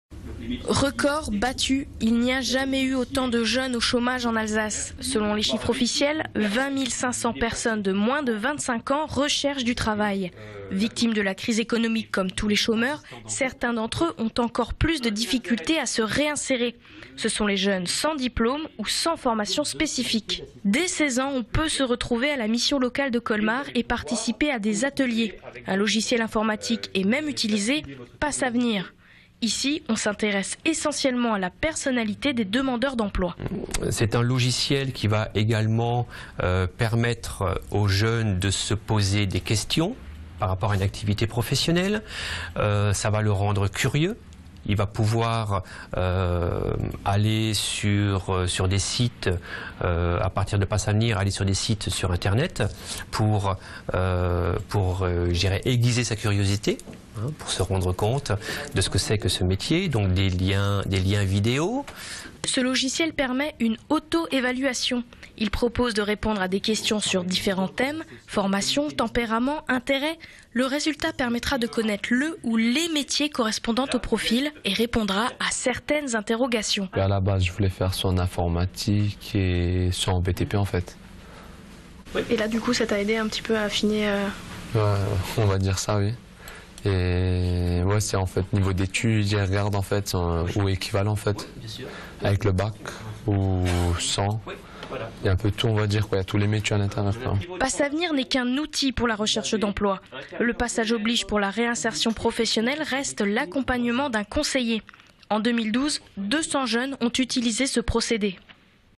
La mission locale de Colmar s'investit pour les jeunes
Sites webs/blogs, copiez cette vidéo avec le bouton "Intégrer" !
 La mission locale de Colmar propose de l'aide aux jeunes qui peinent à trouver du travail.